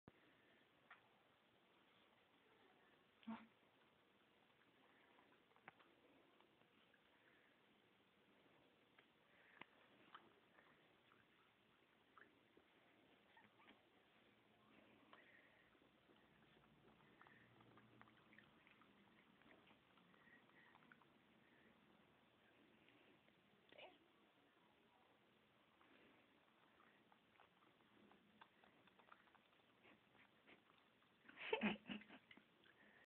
Cute Lizy chewing gum!
That's my cute lhasa apso with 3 years old :)